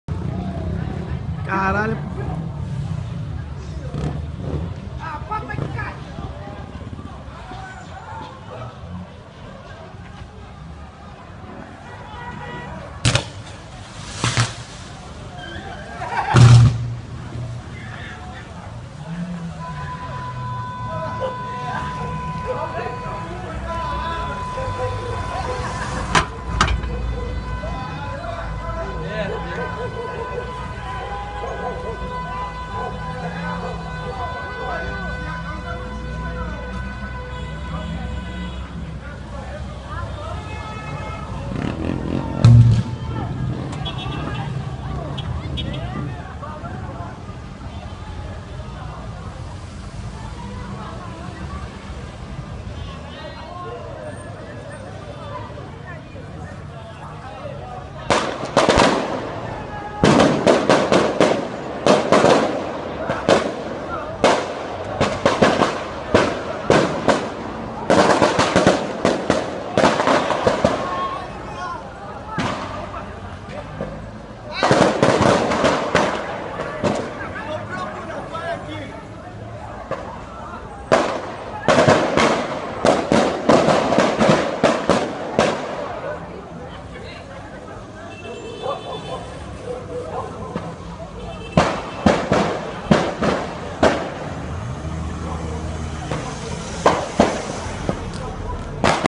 O Perigo de Soltar Balão, esse caiu na Rua Moura, esquina com a Arthur Rios
Campo Grande Rio de janeiro